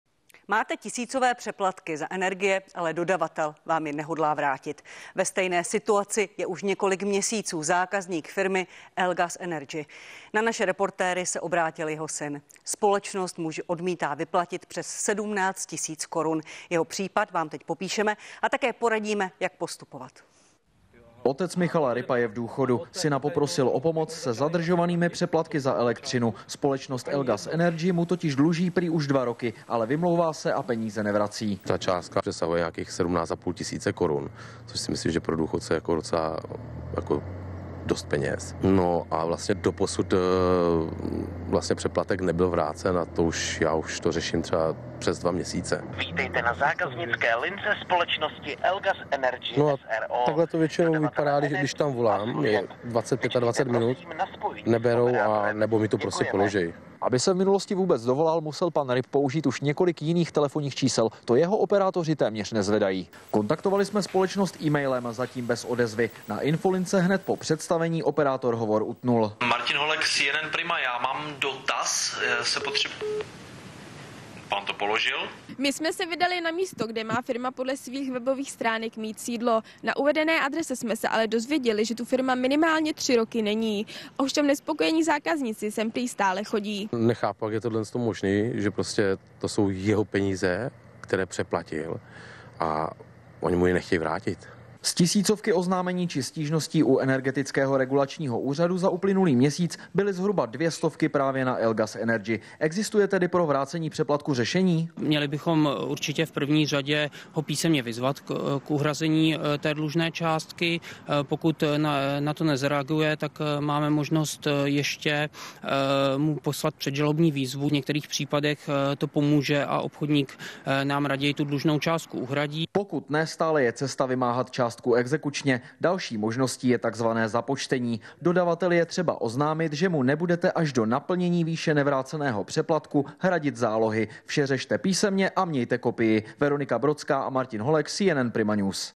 Dodavatel dluží zoufalému seniorovi 17,5 tisíce na přeplatcích za energie. Jak postupovat?
Máte tisícové přeplatky za energie, ale dodavatelská společnost vám je nehodlá vrátit? Právě takový případ už přes dva měsíce řeší pan Ryp se společností Elgas Energy, která mu odmítá vyplatit přes 17 tisíc korun. Celý článek ZDE